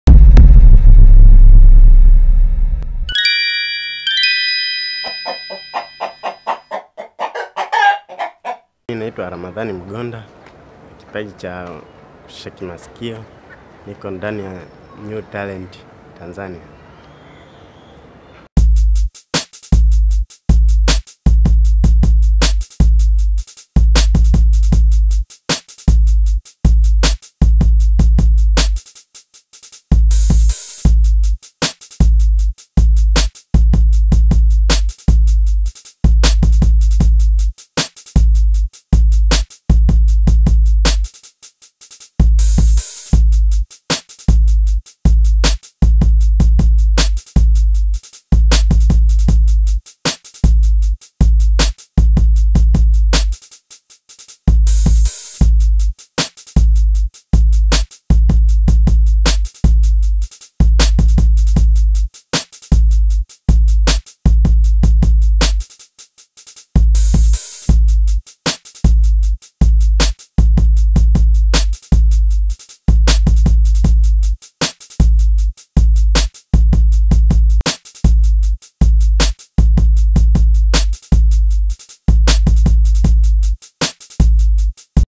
Video: kijana mwenye kipaji cha ku- shake masikio kama mbuzi!
Duniani tumeumbiwa mengi sana kila mtu huzaliwa na kipaji chake ambacho sio cha kujifunza, sasa basi leo tumekuletea Kipaji Cha kijana ambaye anatambulika kama Ramadhan Mgonda, yeye anakipaji cha Ku- shake masikio yake kama mbuzi.